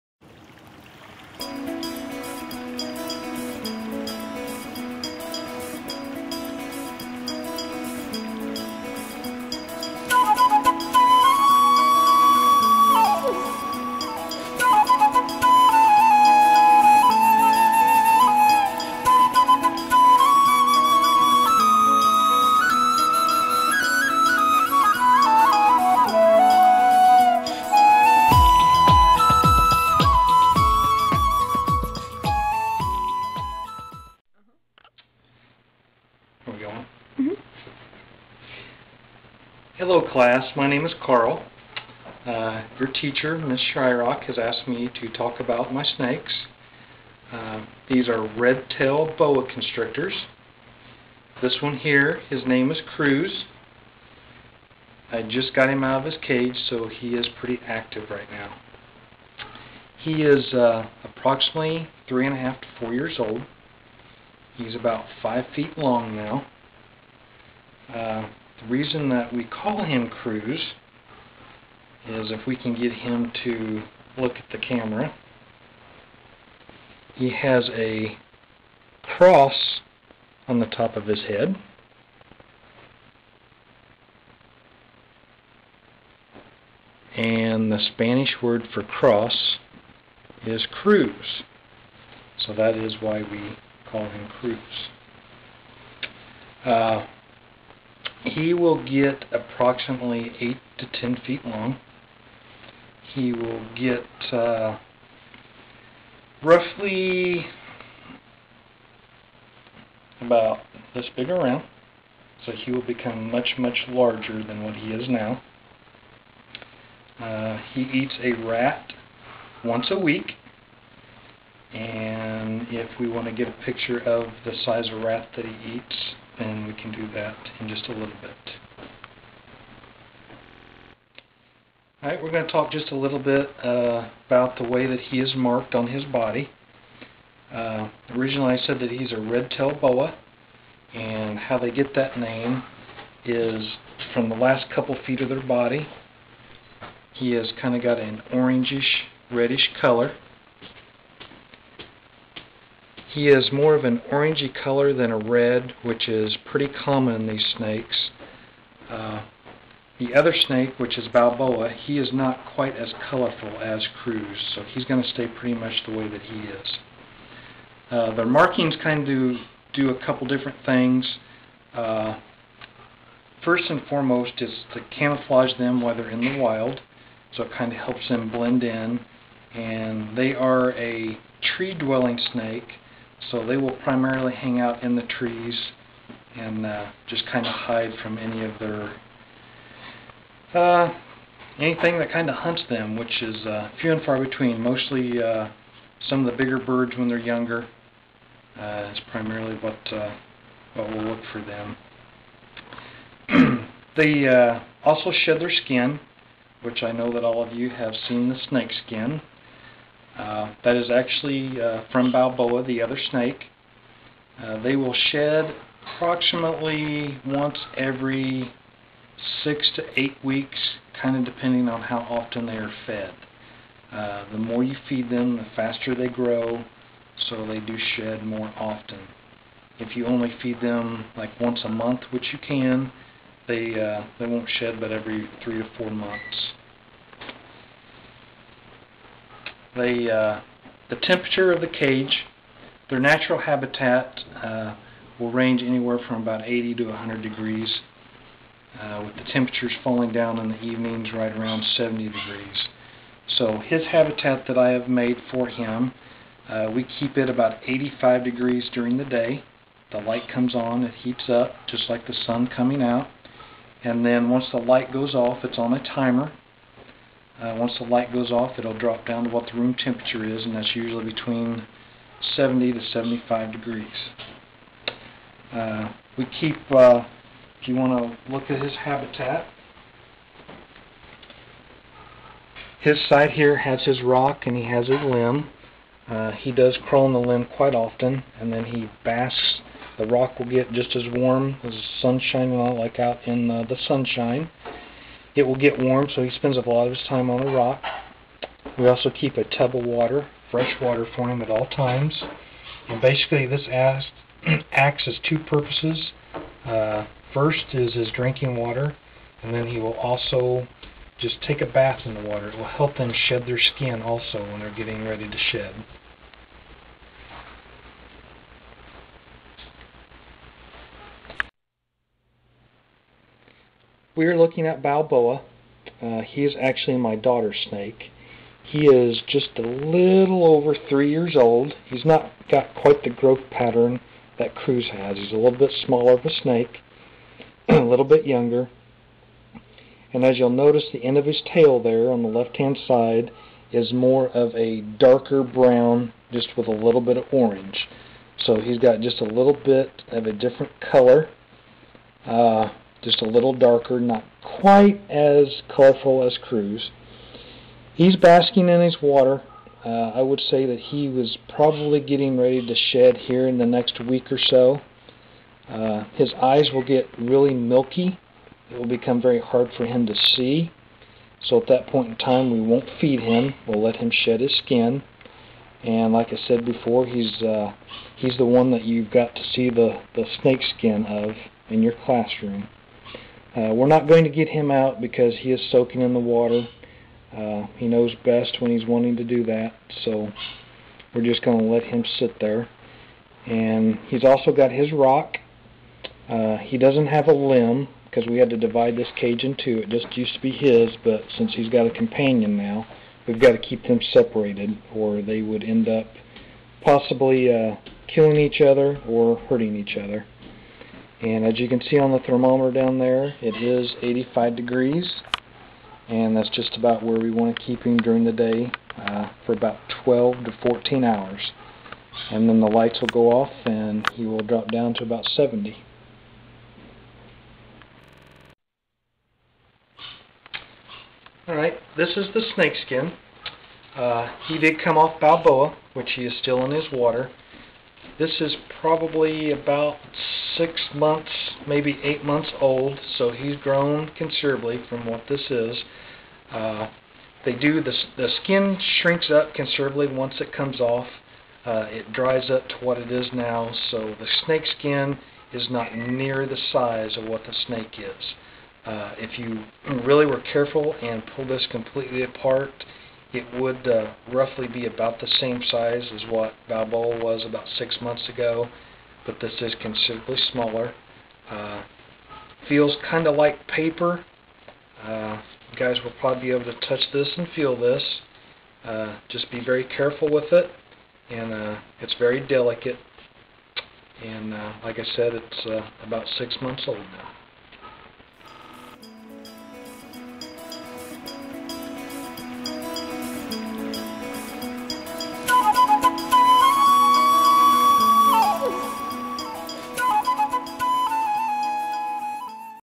Red-Tailed Boa Constrictors
I created this video so my students could learn more about snakes since we were not able to bring a live snake to the classroom.